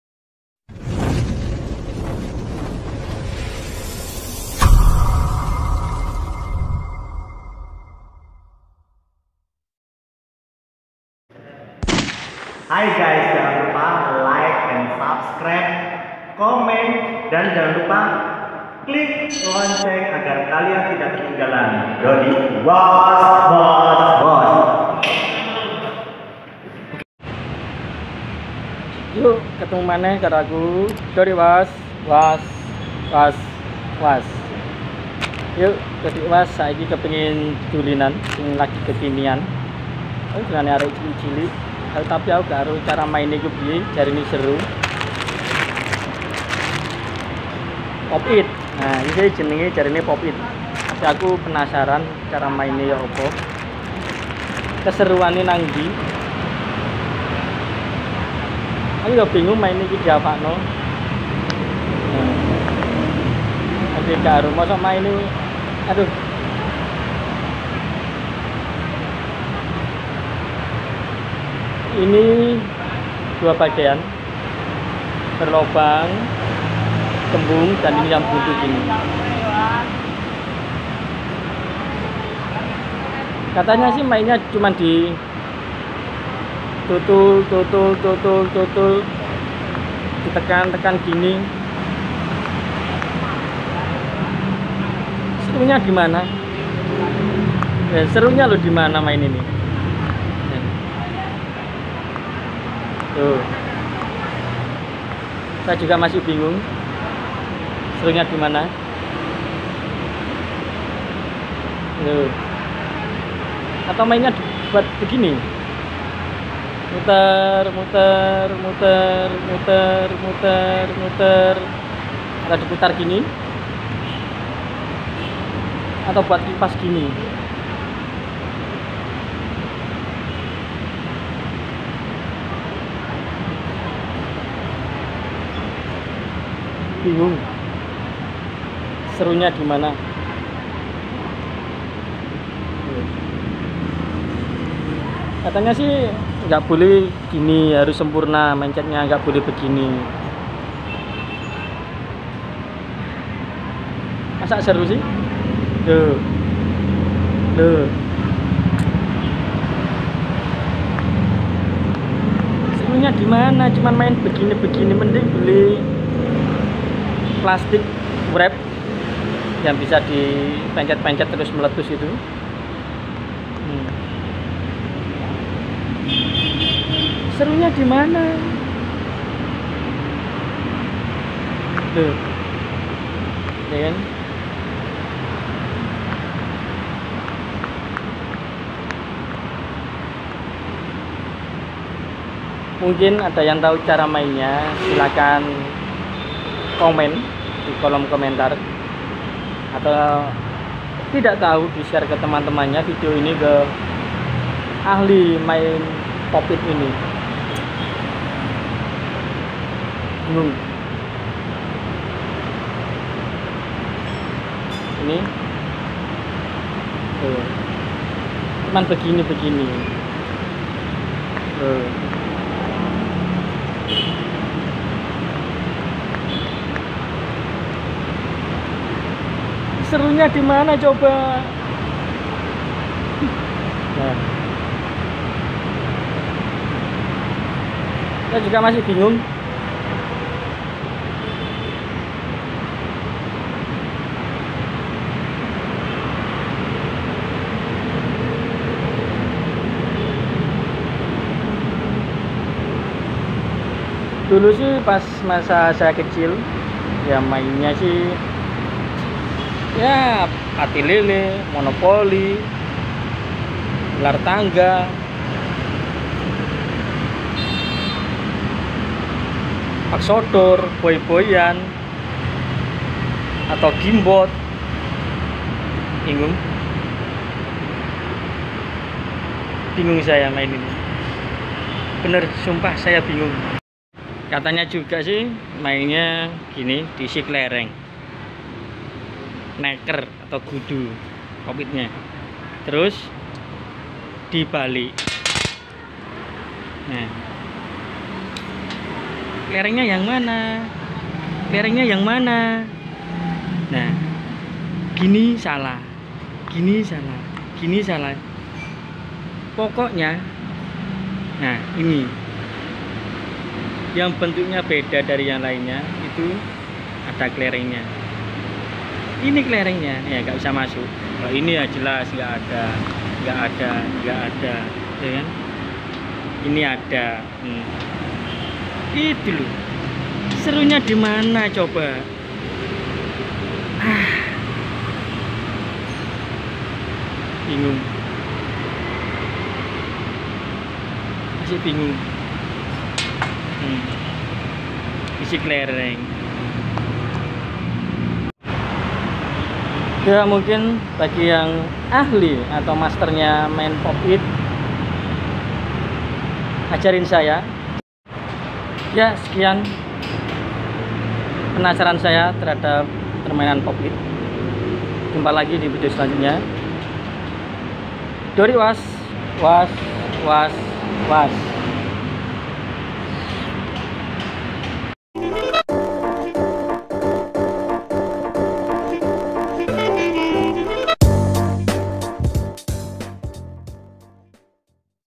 POP IT !!! Mainan Viral Kekinian
POP IT,,Mainan Kekinian Yang Digemari Anak - Anak Kecil Sekarang Ini Viral.Mainan Dari Karet Silicone Yang Bermacam Bentuk Dan Warna - Warni